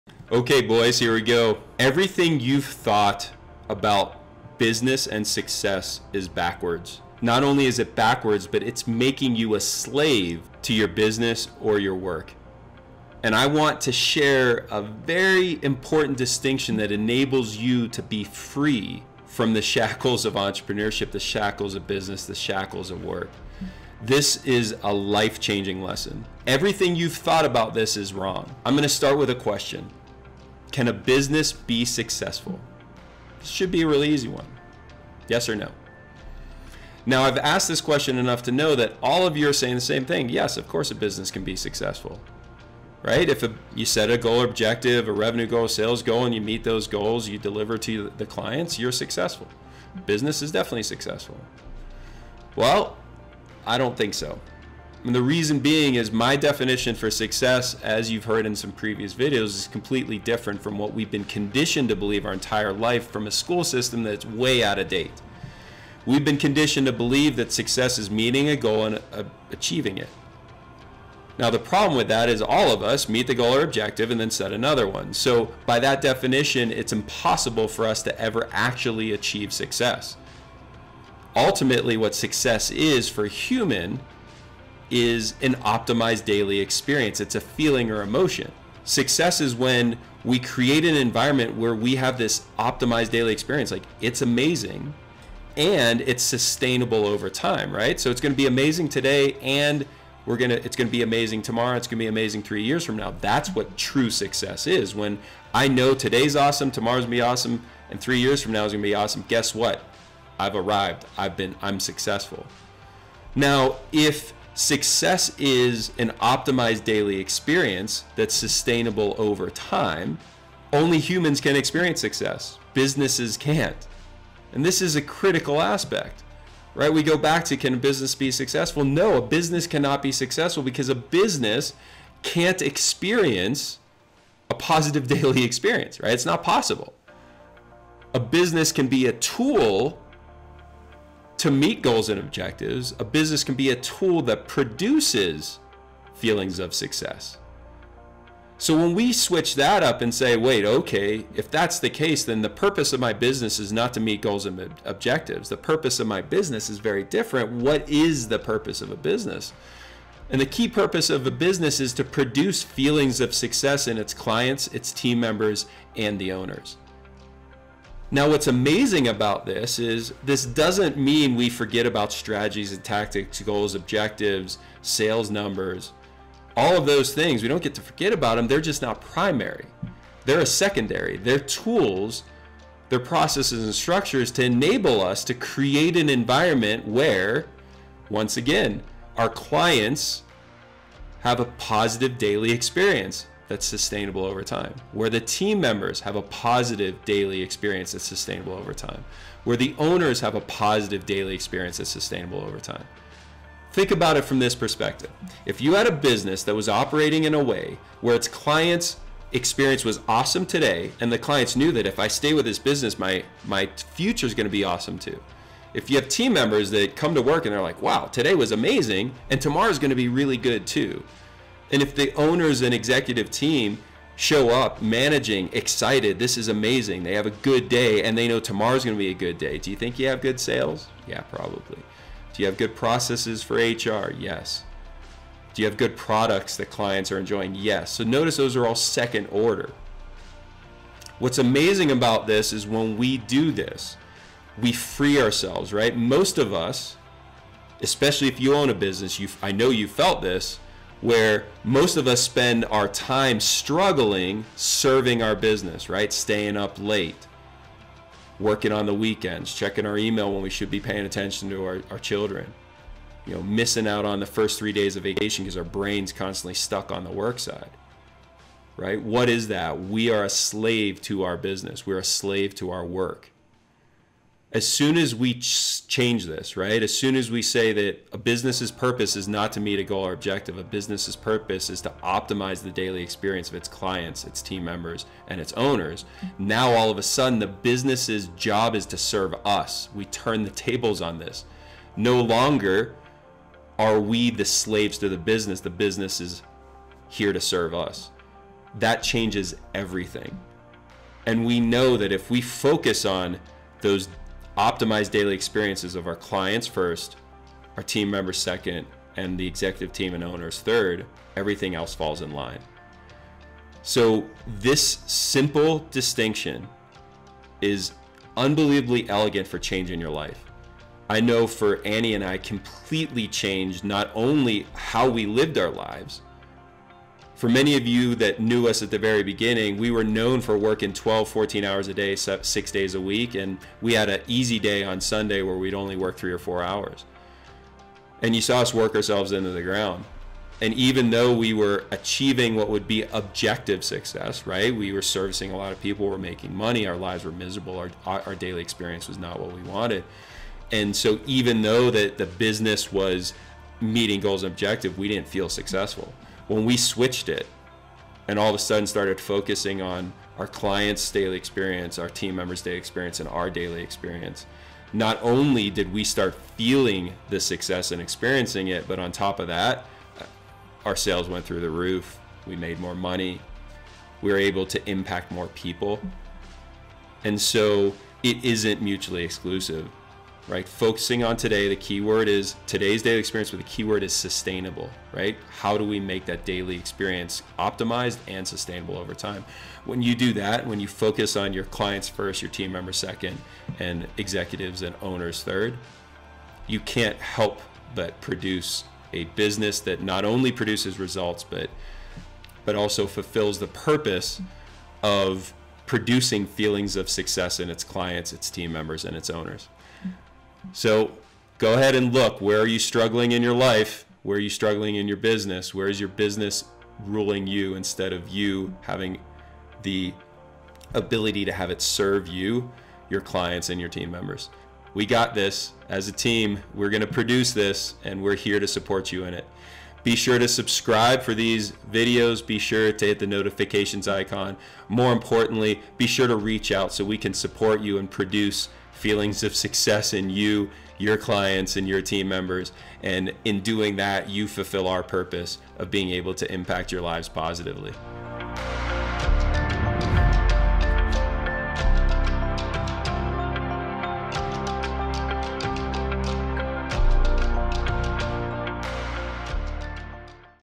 Why Your Business Will Never Be Successful
I am pretty darn sure that everything you've been taught about business and success is
backward.

Not only is it backwards, but it's making you a slave to your business.

I want to share a very important distinction that enables you to be more free...

- from the shackles of entrepreneurship
-from the shackles of business
-from the shackles of work

Once again, I am sharing a life-changing lesson and I'm going to start with a question.

Can a business be successful?

I've asked this question enough to know that all of you are saying “yes, of course!”

You are thinking that a business can be successful because the leaders set goals and objectives, and the company meets those goals….. Right?

This answer has been conditioned in each of us over a lifetime.

And this answer is completely BACKWARDS.

What if I told you that you could experience success, right now?

Watch my video, you won’t be disappointed.